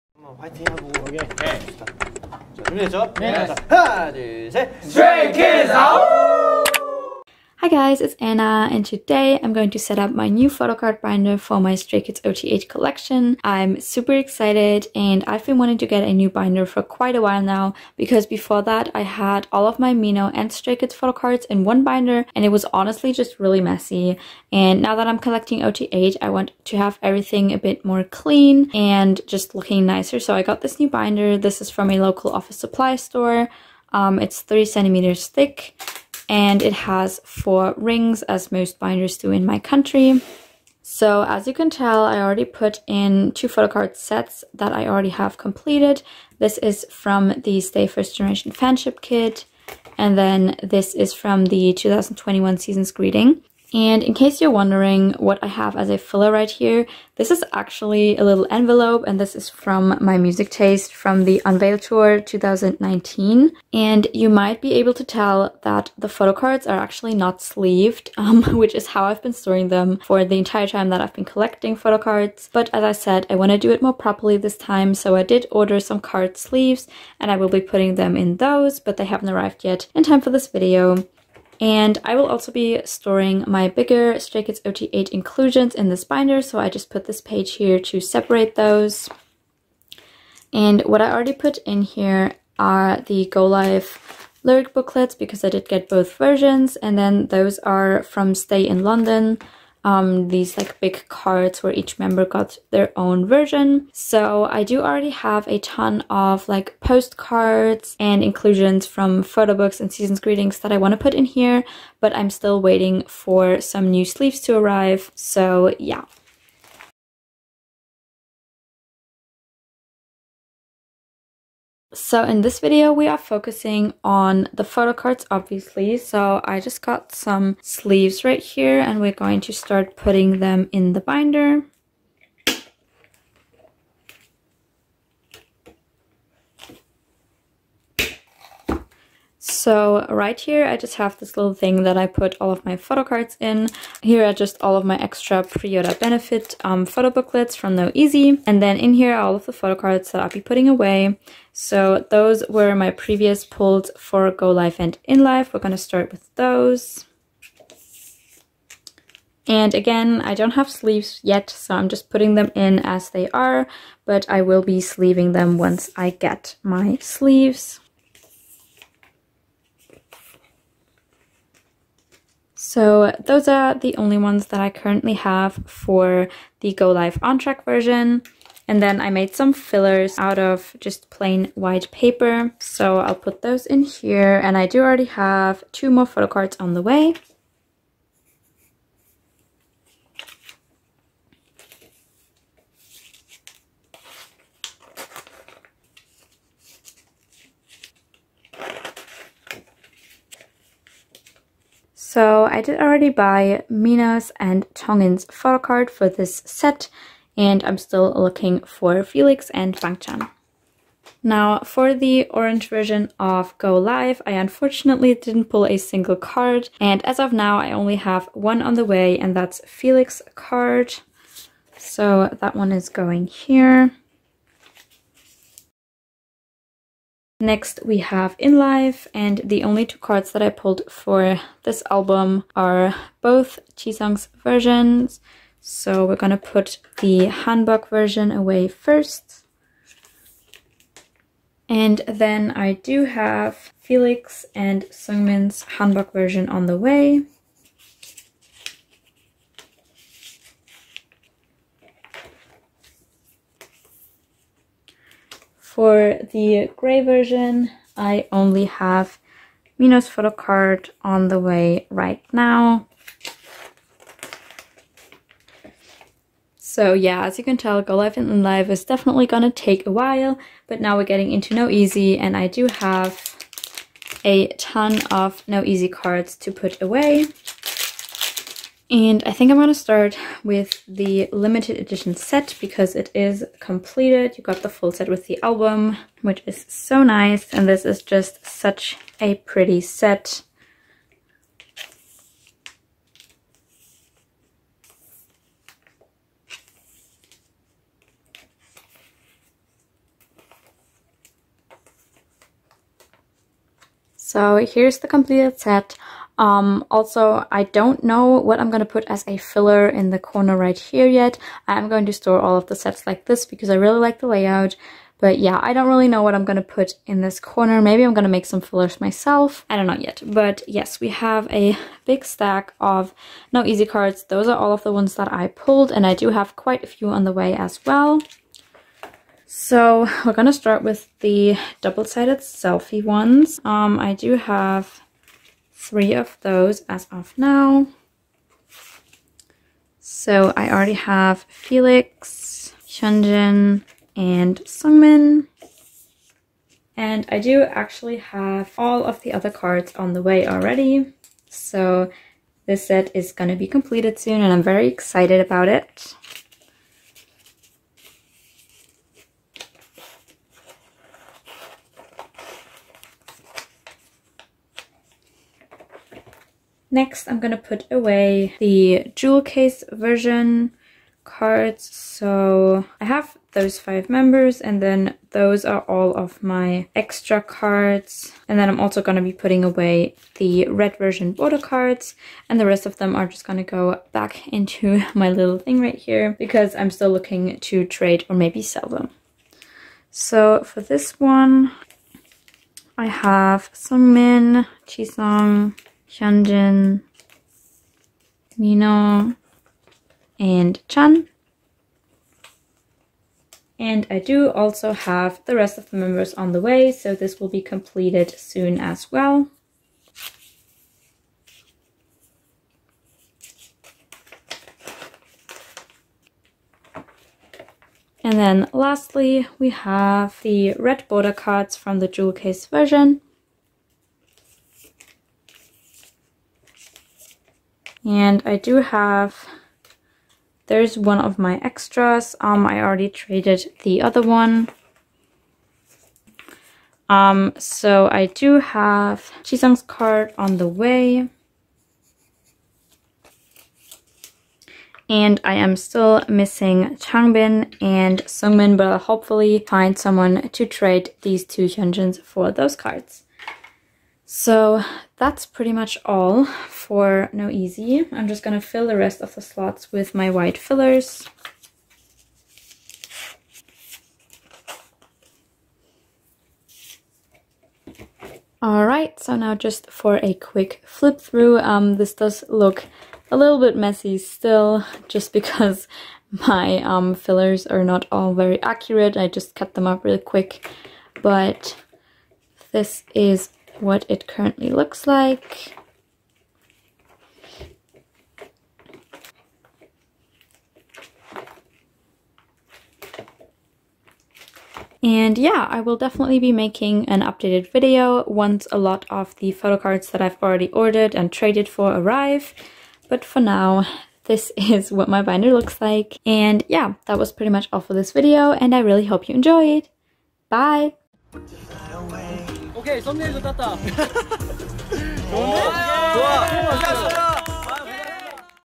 setting up a new photocard binder for my stray kids ot8 collection! ✧
trade/sale twitter: @lovstaysale